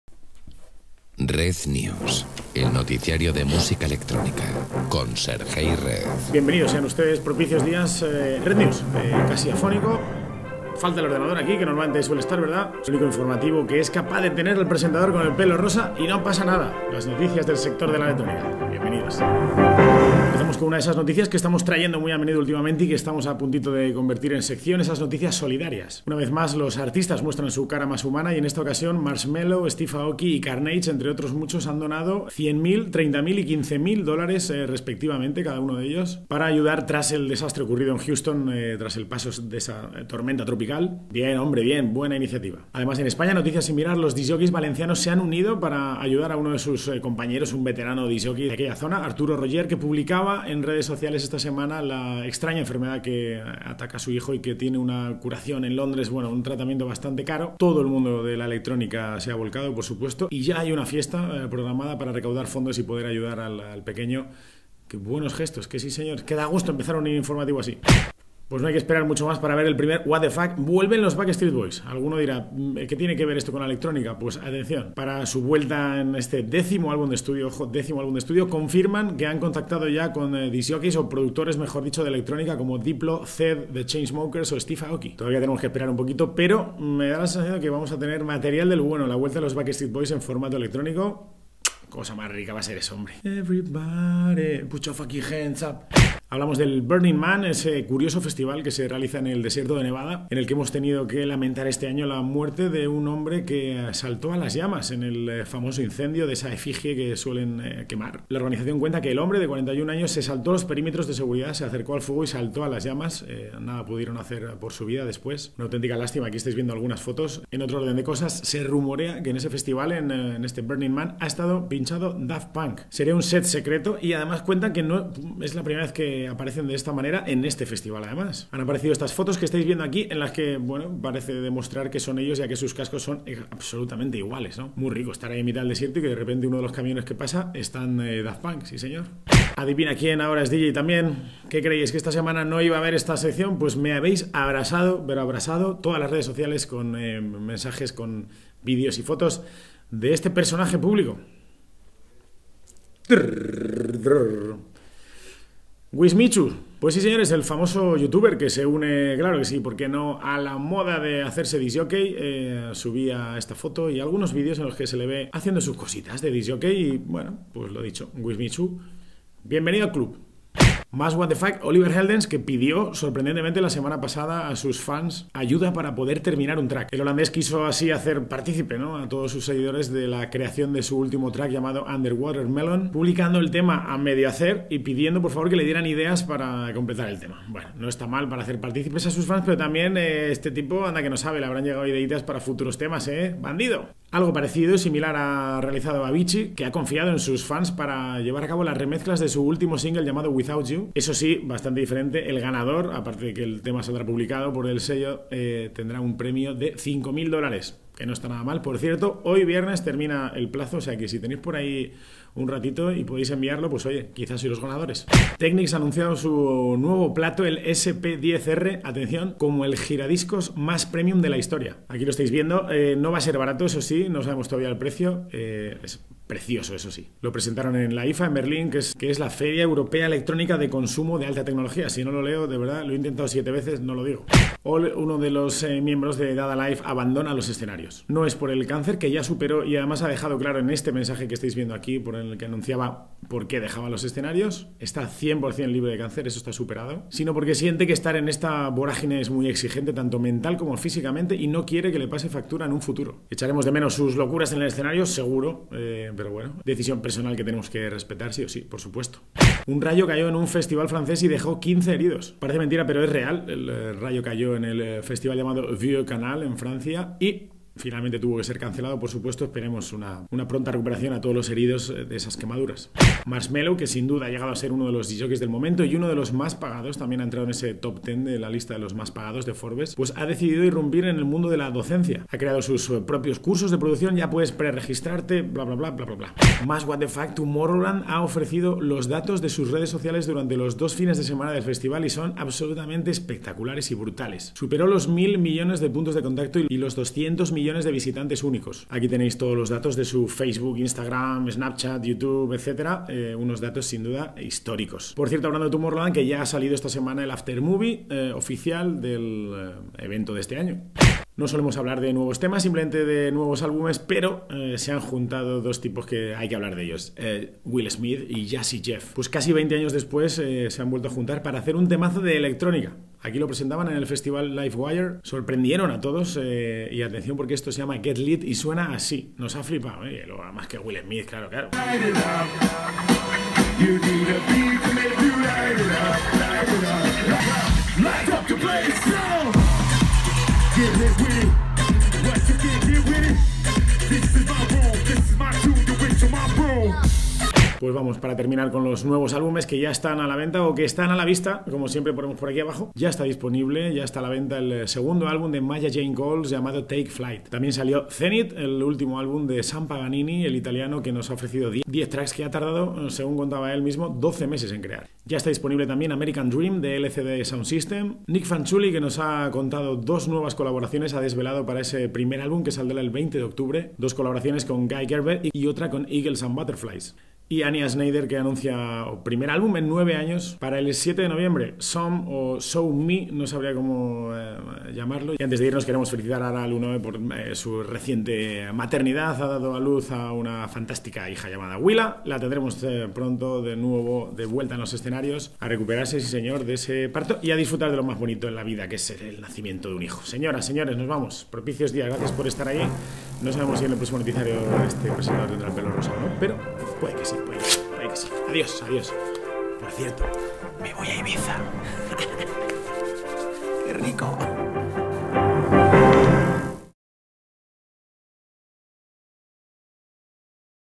REZ NEWS [08.SEP.2017] Noticiario música electrónica #042
Tarde pero llegamos señoraaa!!
- Adivina quién ahora es DJ también: un muuuy famoso youtuber
- Technics presenta "el plato más premium de la historia"
- WTF! Oliver Heldens y Avicii te necesitan para su música
- WTF! Vuelven los Backstreet Boys y colaborarán con muchos djs
- Anna Lunoe ha sido mamá... enhorabuena!
- Un rayo cae en un festival francés
- Un fallecido en el festival Burning Man
- Los DJs, más solidarios que nunca
- Olle de Dada Life abandona los escenarios :(
- Marshmello debuta como profesor en sus cursos de producción.
- Will Smith & Jazzy Jeff vuelven 20 años después con un tema de electrónica
- Nuevos álbumes a la vista: Maya Jane Coles, Sam Paganini, LCD Soundsystem, Nic Fanciulli, Anja Schneider